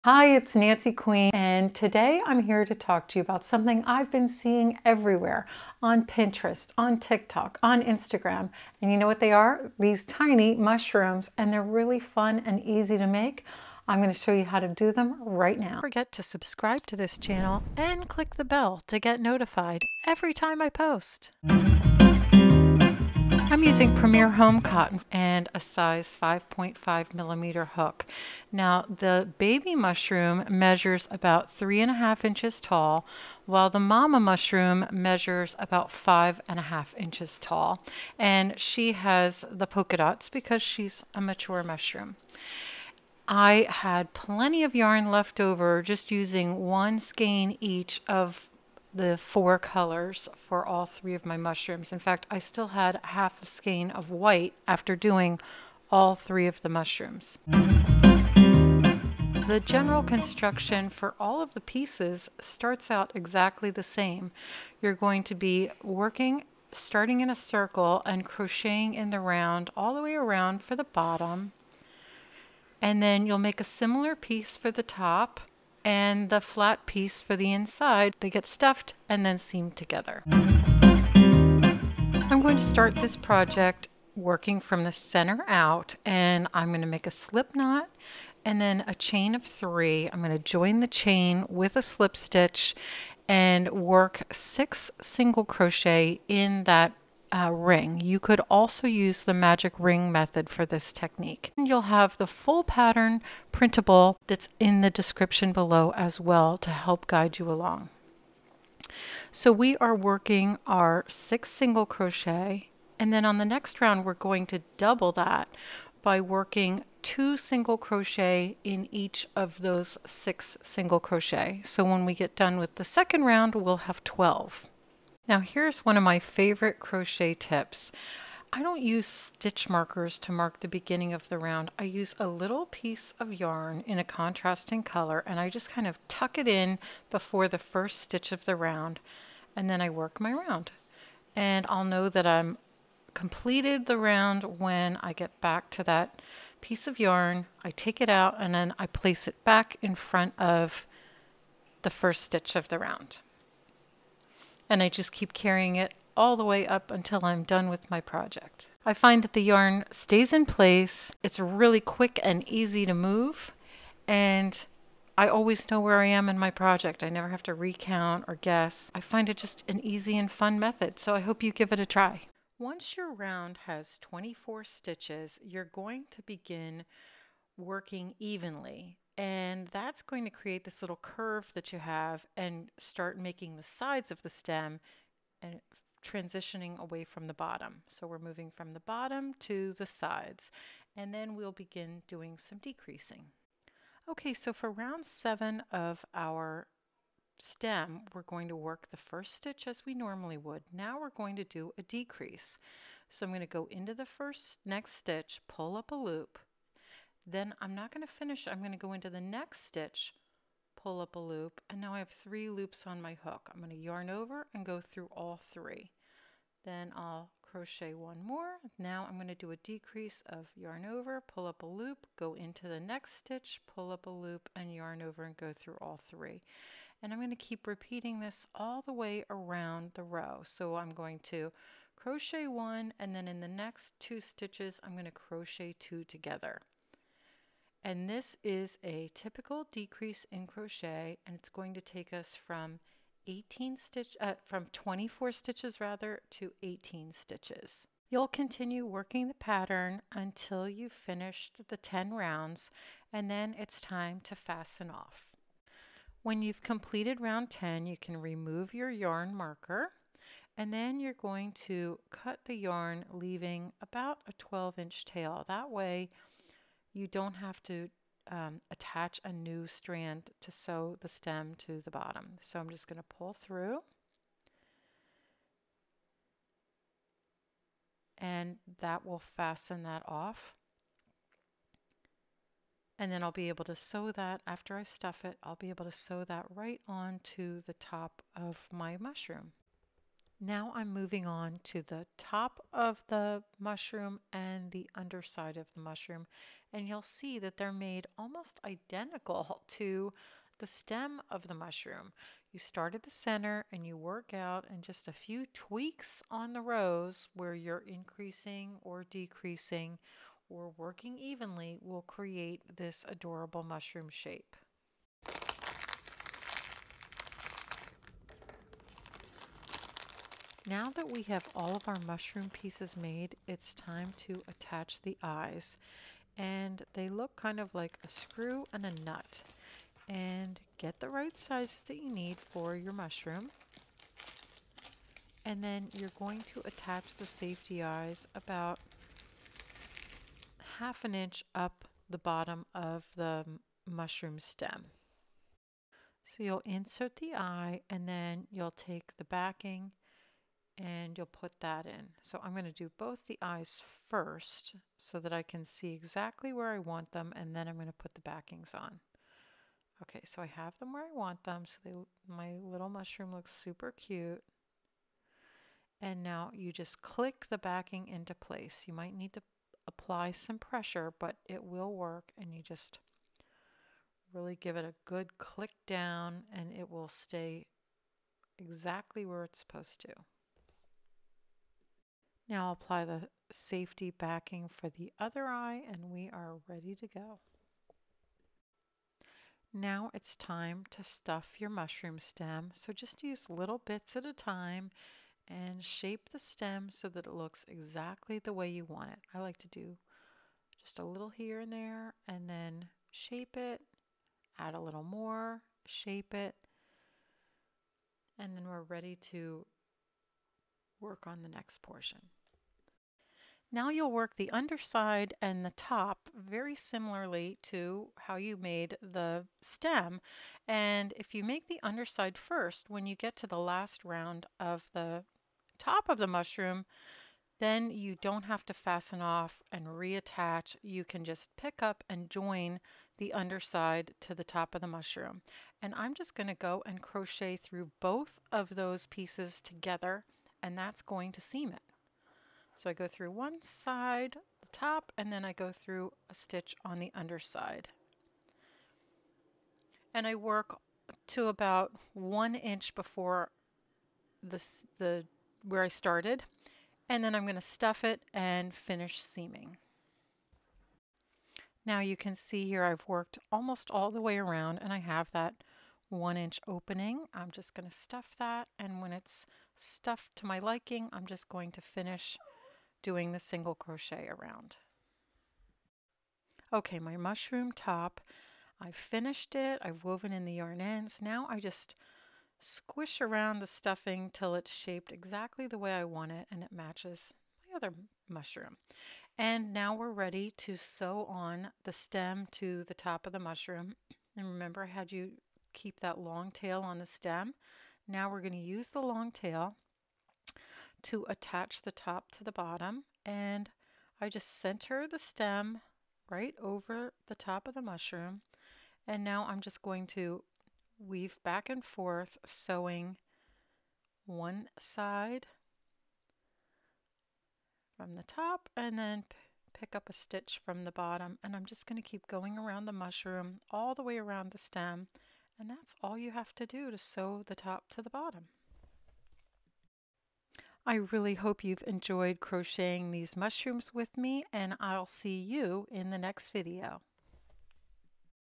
How to Crochet Cute Mushrooms | Beginner Crochet DIY
Mushrooms are a hot trend! Now you can crochet a tiny mushroom family with this mushroom plushie stuffed toy tutorial and free crochet pattern. These mushroom stuffies are easy to crochet and suitable for beginners.

Pattern includes two sizes: Baby (Mama)
Completed Measurements: 3.5" (5.5)" tall

MATERIALS
1 skein each of 2 colors Premier Home Cotton Yarn (or worsted weight #4 yarn)
Size H/8 (5.5mm) crochet hook
Finishing needle
Scissors
Polyfil
2 black safety eyes: baby 8mm (mama 12mm)

NobleKnits/Nancy Queen
1048 Irvine Avenue, PMB 323